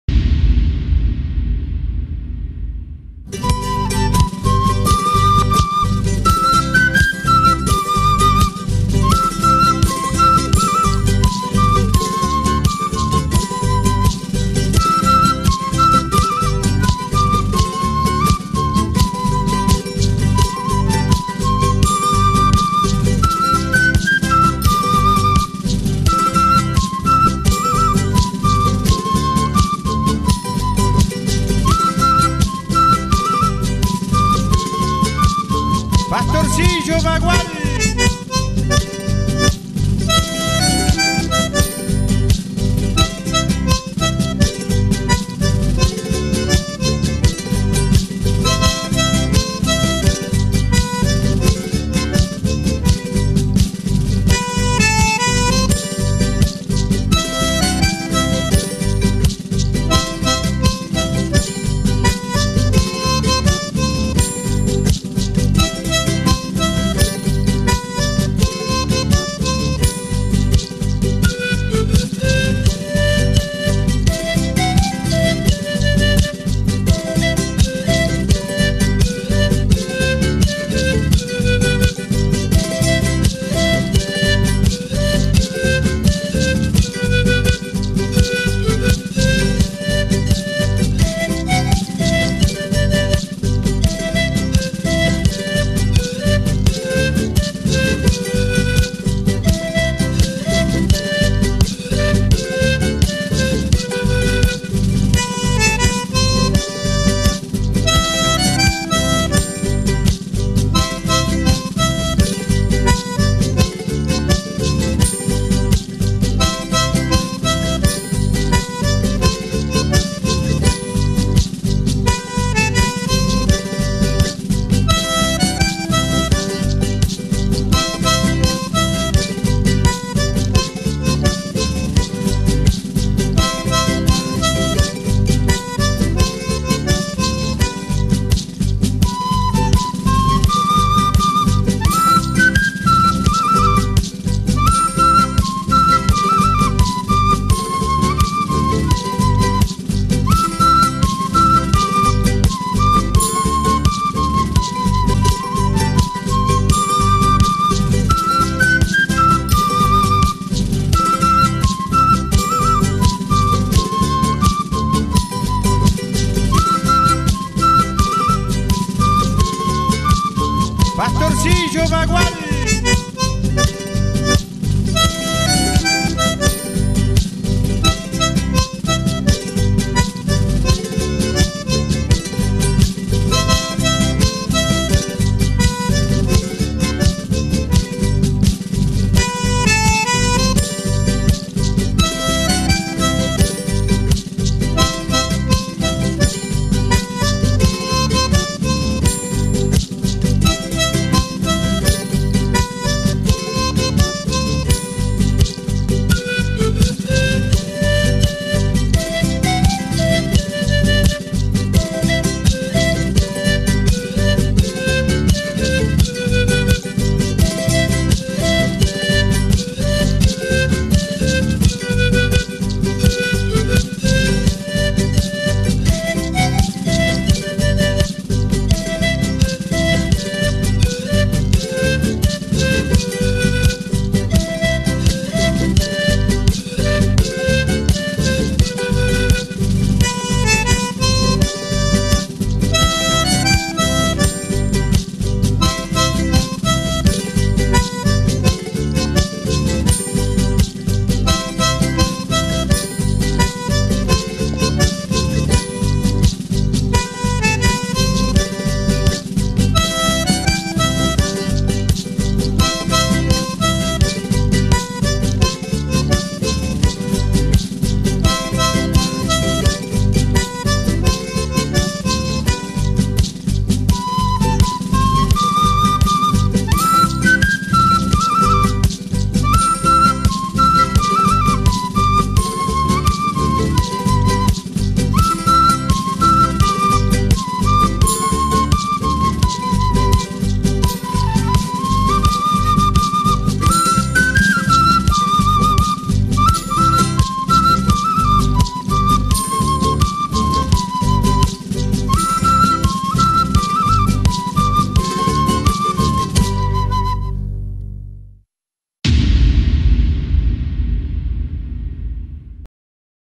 Adoración 1 (La Quiaca Enero De 2010)
ADORACIÓN: en el Noroeste Argentino, se emplea el término para hacer referencia a diversas expresiones coreográficas en torno a la figura del Niño Jesús.